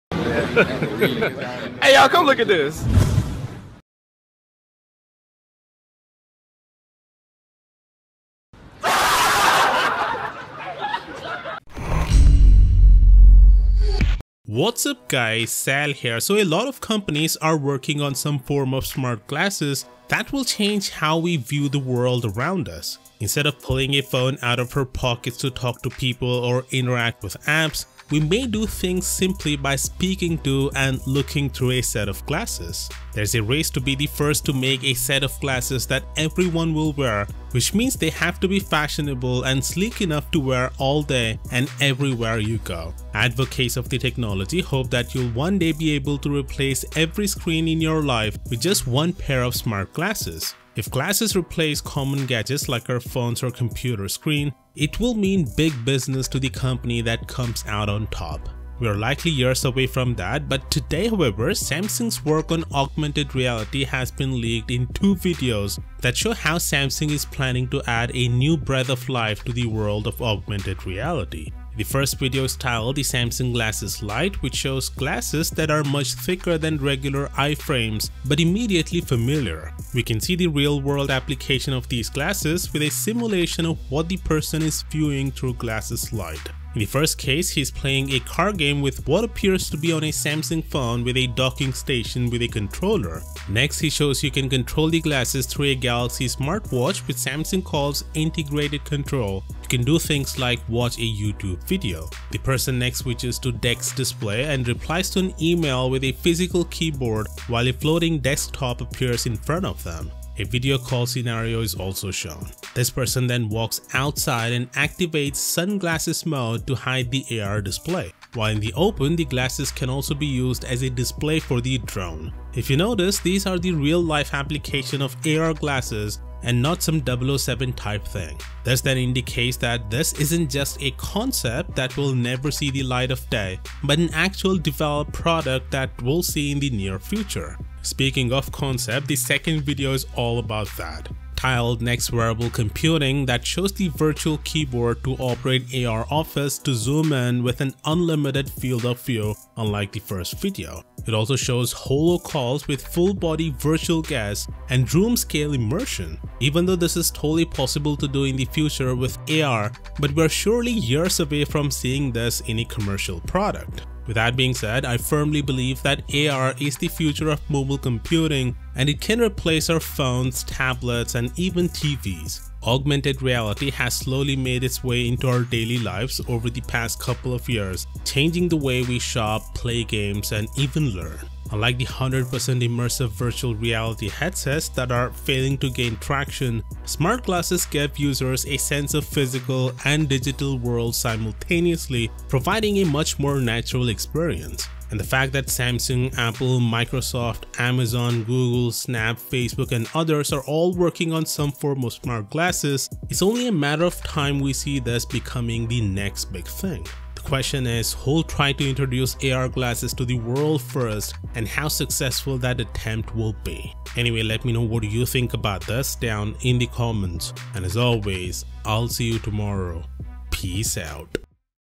Samsung - FIRST LOOK At The Future
Samsung is working on AR glasses and we have two official videos demonstrating what we can expect from Samsung when it comes to Augmented reality. The first video is titled Samsung Glasses Lite that shows the real-world application of AR glasses while the second video is more of a Concept. No word on when we might see the AR Glasses from Samsung.